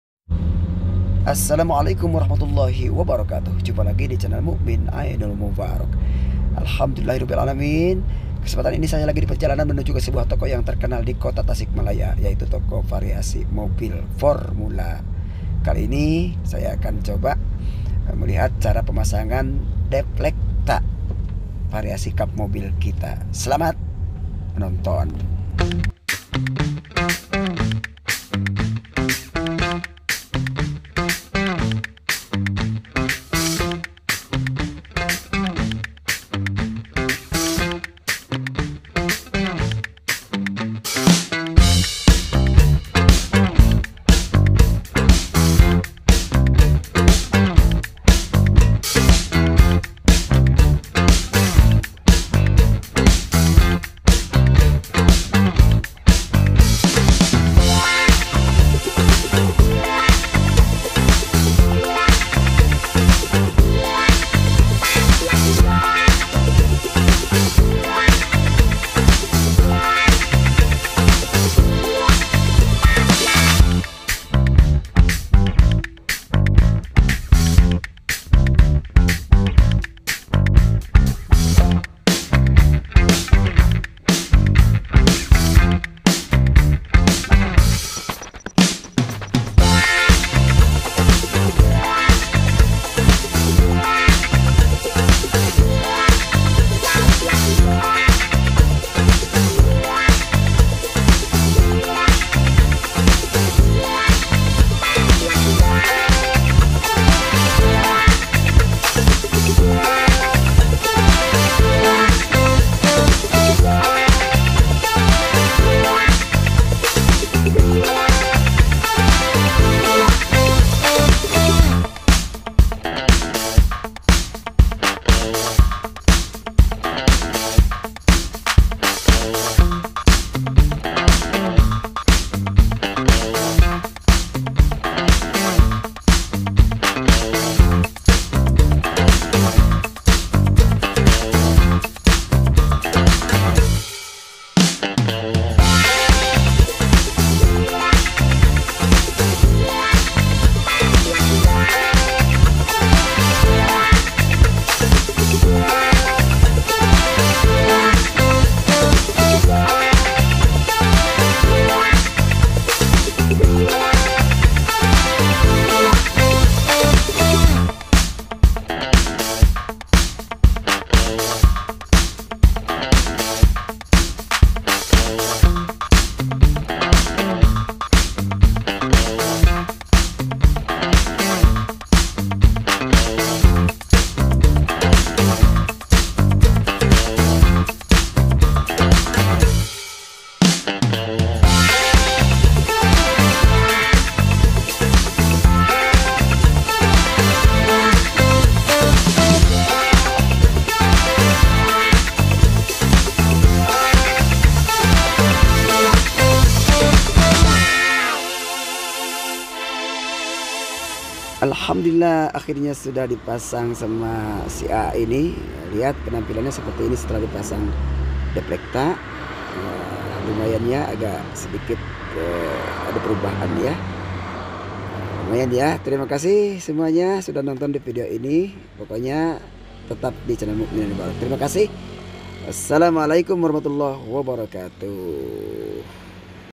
full variasi lagi pasang deflecta mobil fortuner jadi keren aman
gunanya pasang deflecta ini buat melindungi goresan kap mobil anda
semoga bermanfaat amiin